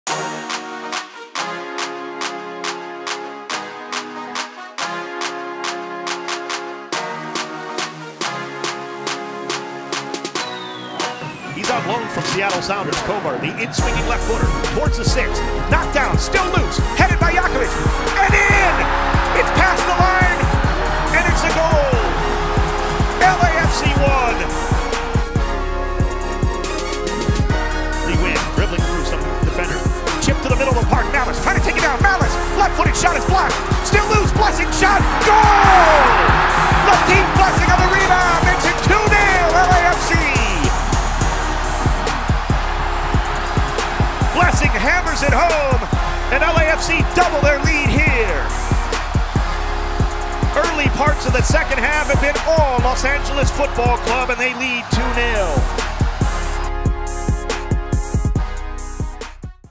Full Highlights: LAFC vs. Fresno FC | U.S. Open Cup 4th Round
Highlights from our first-ever Lamar Hunt U.S. Open Cup match, a 2-0 win against Fresno FC at Banc of California Stadium.